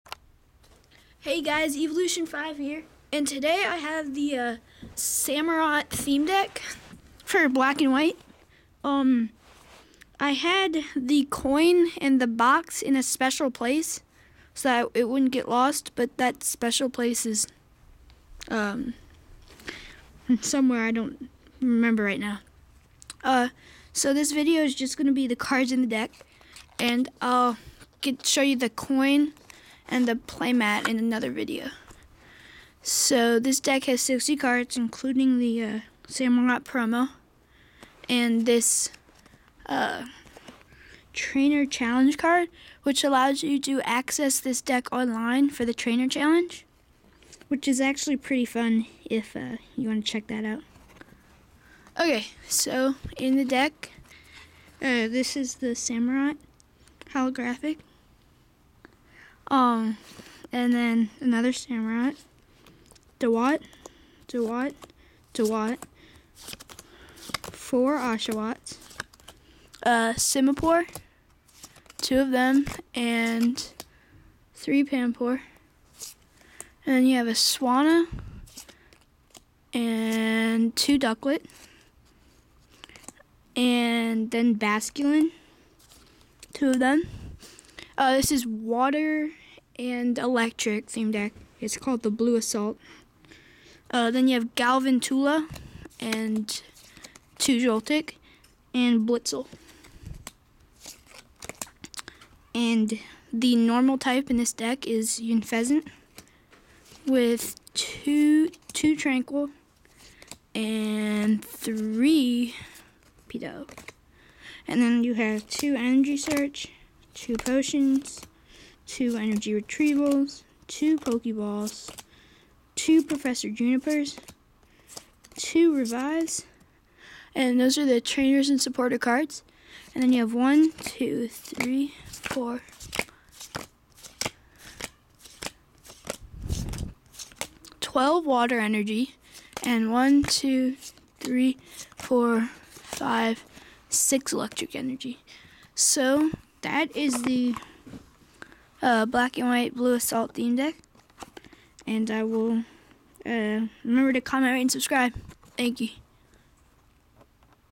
Black and White Theme Deck-- Blue Assault
We open the Blue Assualt theme deck featuring Samurott. Overall I think that it is a pretty good deck, but I actually got this before the set came out and really just wanted to see some of the cards. Good cards though!